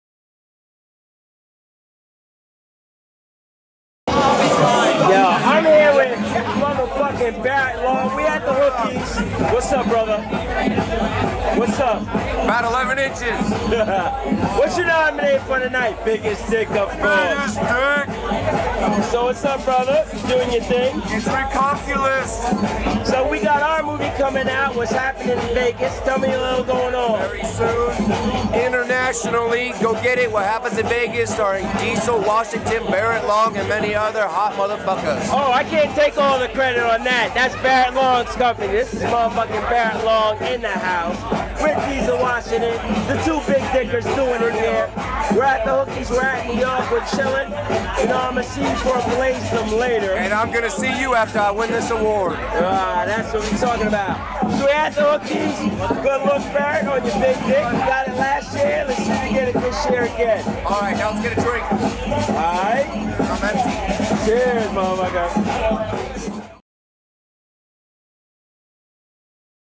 Diesel Washington Interviews
Diesel Washington interviews Barrett Long.